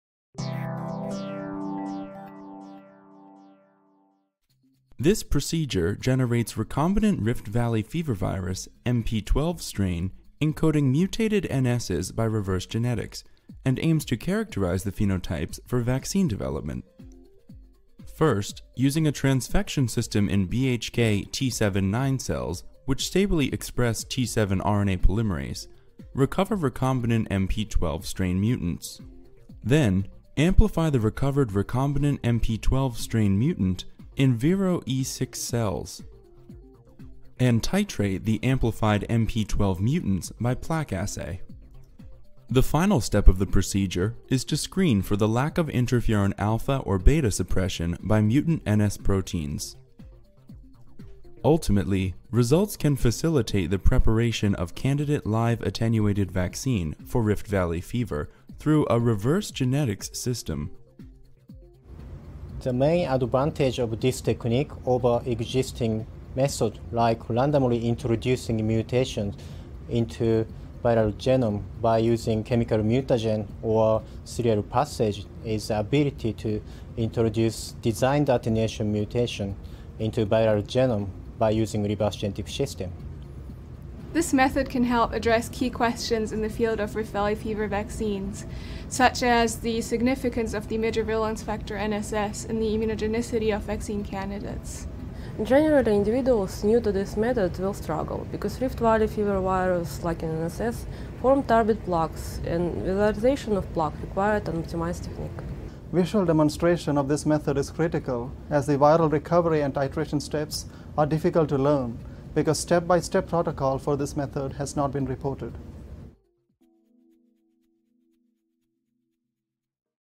Reverse Genetics: Rift Valley Fever Virus MP-12 Strain: Vaccine Safety & Efficacy l Protocol Preview
Using Reverse Genetics to Manipulate the NSs Gene of the Rift Valley Fever Virus MP-12 Strain to Improve Vaccine Safety and Efficacy -  a 2 minute Preview of the Experimental Protocol

Birte Kalveram, Olga Lihoradova, Sabarish V. Indran, Tetsuro Ikegami
University of Texas Medical Branch, Department of Pathology;

The reverse genetics system for the Rift Valley fever virus MP-12 vaccine strain is a useful tool for creating additional MP-12 mutants with increased attenuation and immunogenicity. We describe the protocol to generate and characterize NSs mutant strains.